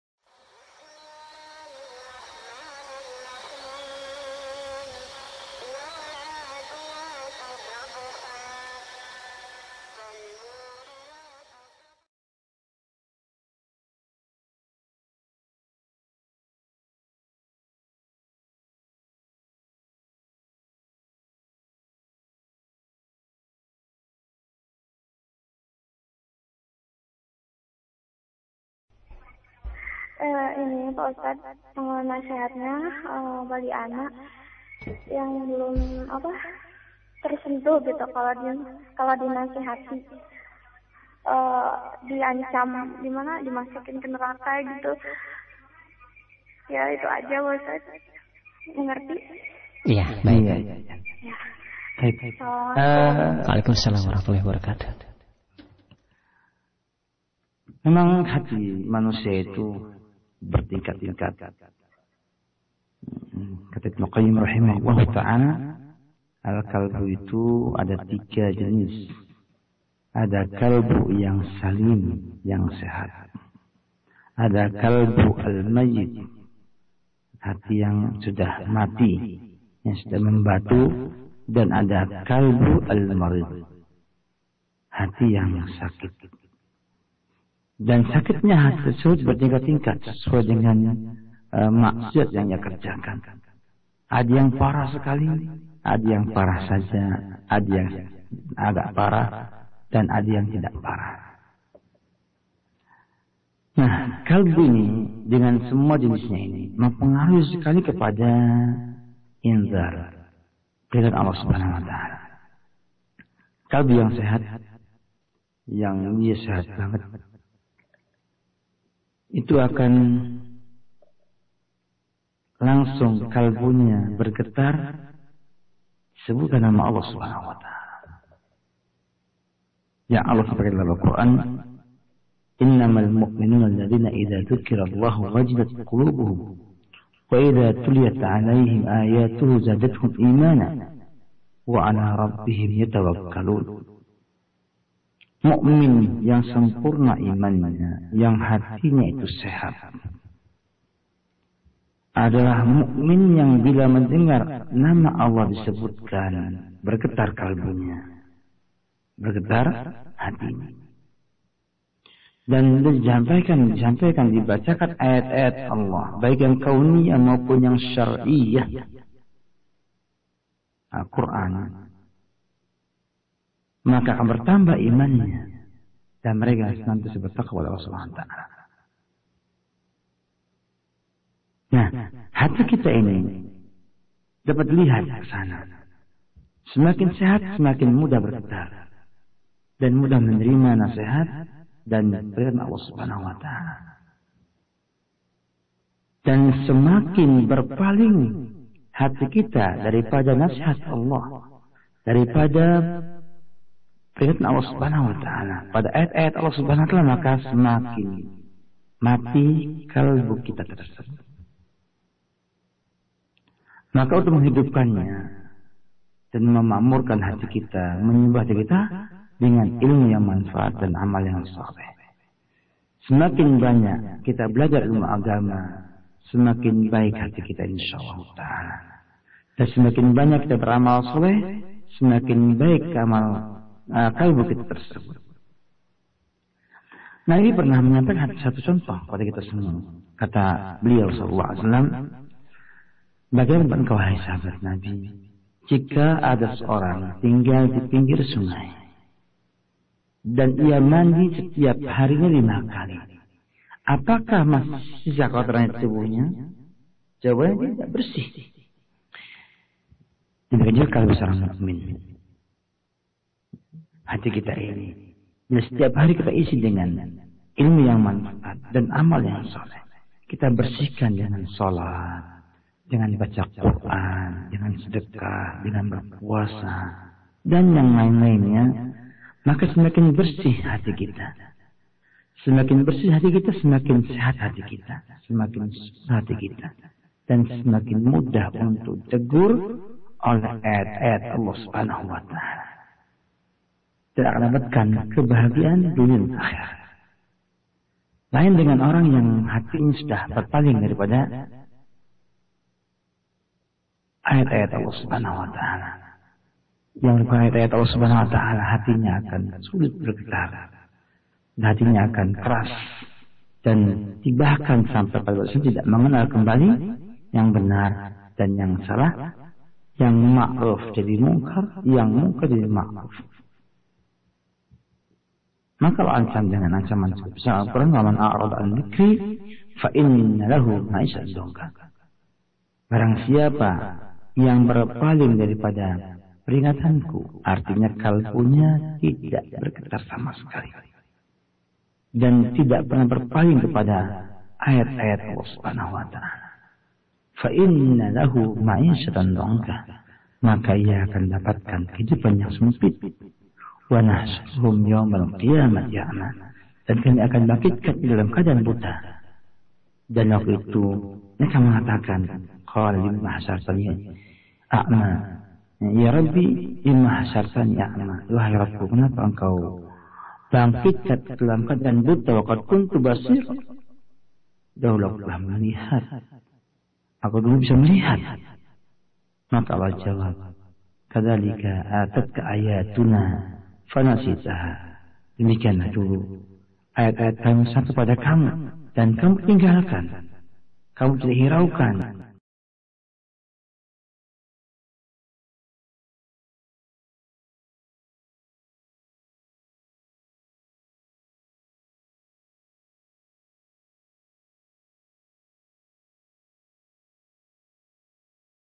Jika Diri Masih Belum Bisa Tersentuh Dengan Dakwah - Ustadz Aunur Rofiq Ghufron ( Satu Jalan Lurus )
Silahkan Hubungi :
IG : asadussahra IG : umrohekspansi